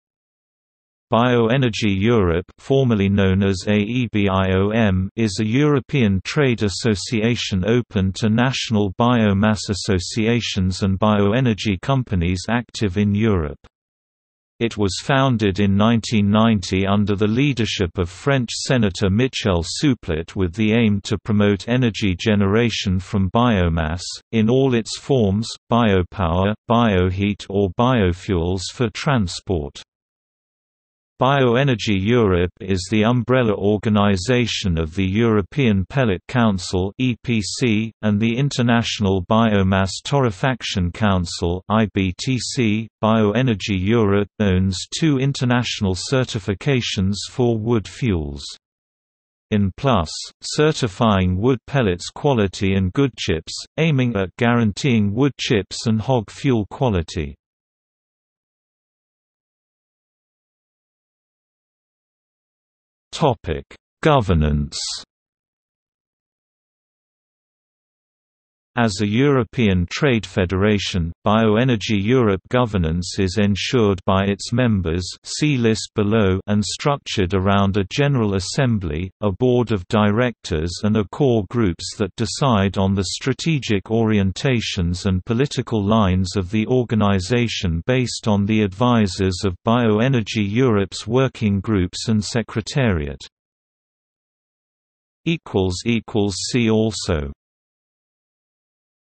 European Biomass Association | Wikipedia audio article
00:00:59 1 Governance

- Socrates

SUMMARY
Bioenergy Europe (formerly known as AEBIOM) is a European trade association open to national biomass associations and bioenergy companies active in Europe. It was founded in 1990 under the leadership of french senator Michel Souplet with the aim to promote energy generation from biomass - in all its forms: biopower, bioheat or biofuels for transport.
Bioenergy Europe is the umbrella organisation of the European Pellet Council (EPC), and the International Biomass Torrefaction Council (IBTC).Bioenergy Europe owns two international certifications for wood fuels. ENplus®, certifying wood pellets quality  and GoodChips®, aiming at guaranteeing wood chips and hog fuel quality.